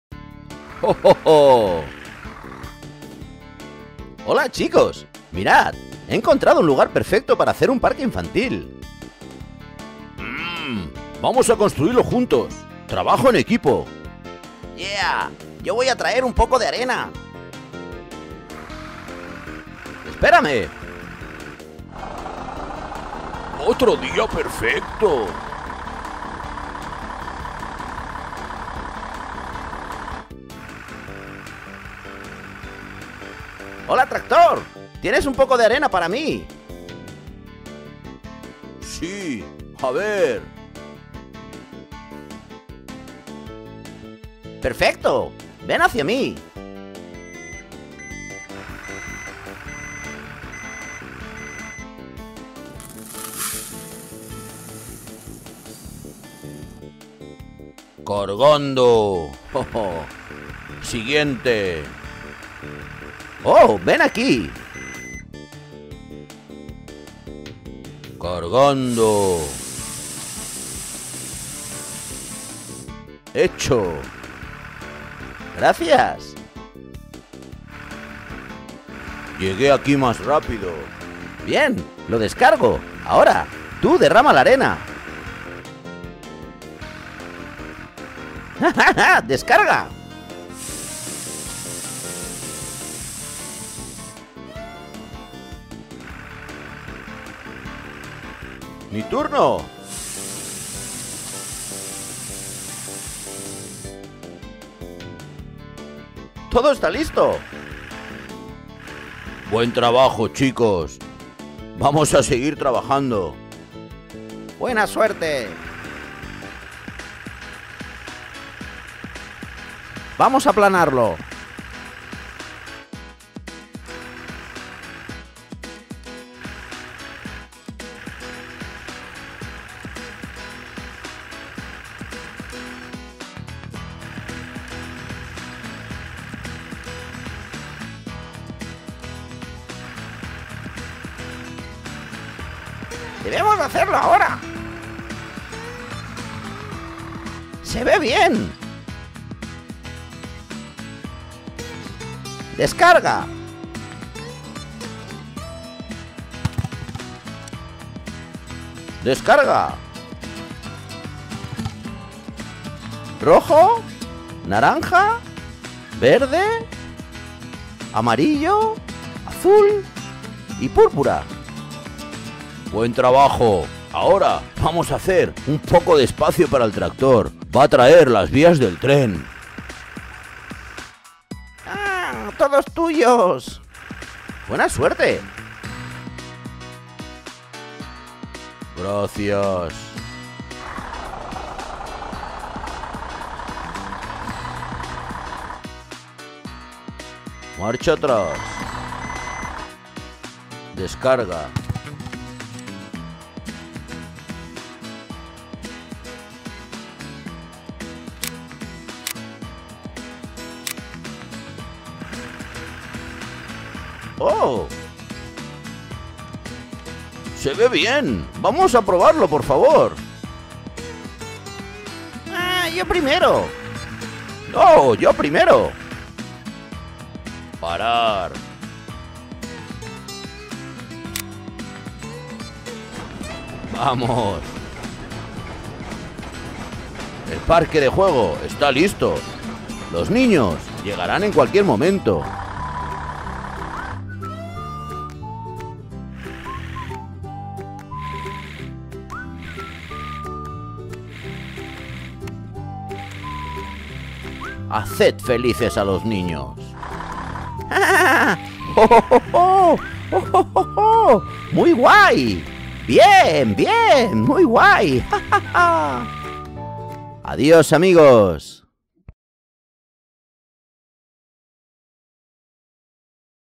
Jugar con coches. Un parque infantil. Videos para niños
¡Vamos a JUGAR con coches, camiones, tractores y trenes juntos!¡Hola chicos, mirad! Un camión ha encontrado un lugar perfecto para hacer un parque infantil. ¡Vamos a construirlo ! ¡Trabajo en equipo! El camión verde va a traer un poco de arena. Un tractor tiene un poco de arena. Cargando ... Siguiente. ¡Todo listo! ¡Buen trabajo chicos ! Vamos a seguir trabajando. ¡Vamos! El parque de juego está listo! Los niños llegarán en cualquier momento! ¡Haced felices a los niños! ¡Muy guay! ¡Bien!